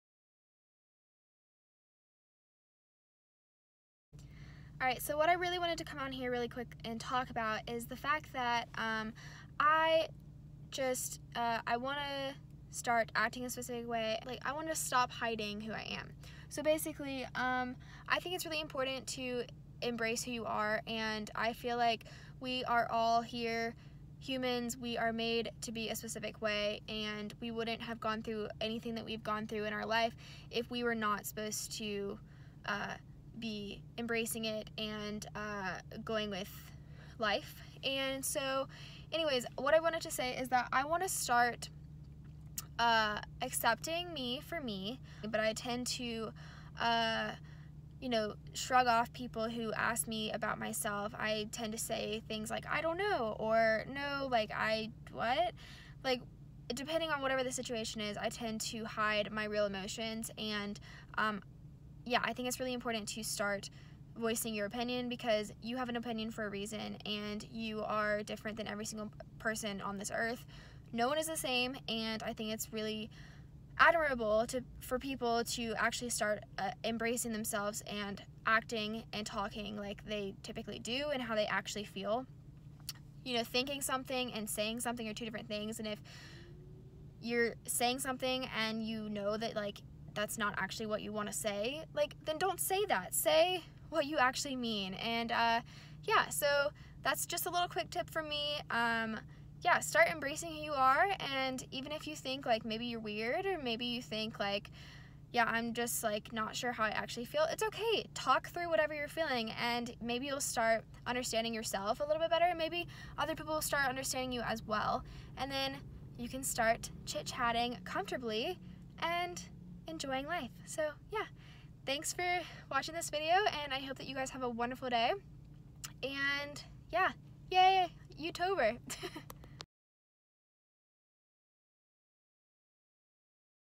Learning to embrace being myself |SELF LOVE
Happy youtober day 16,
It has been a hectic week so far and I have not had much time to create new content. So here is a life chat on the go...I find being yourself to be extremely valuable. I find embracing oneself to be one of the most important things anyone could do EVER. Self love and compassion for oneself is vulnerable and so very needed. I hope you can relate.

XO, Sarah Tess